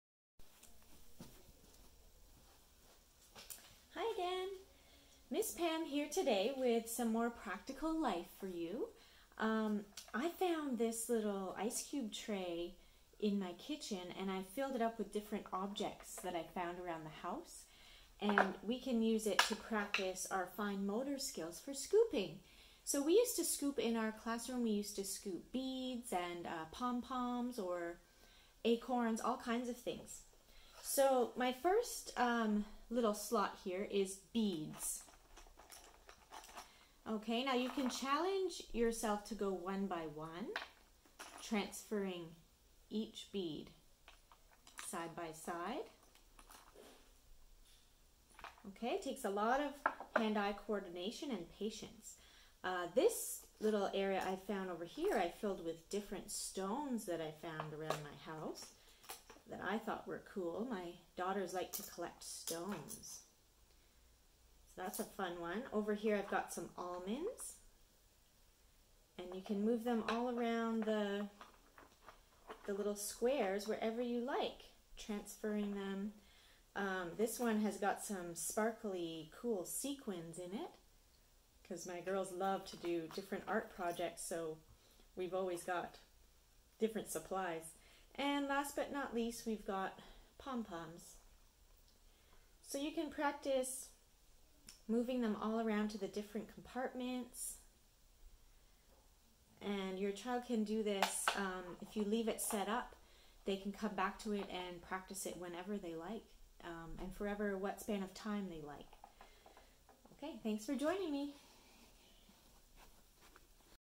Practical Life- Spooning
Use different materials from around the house to practice fine motor development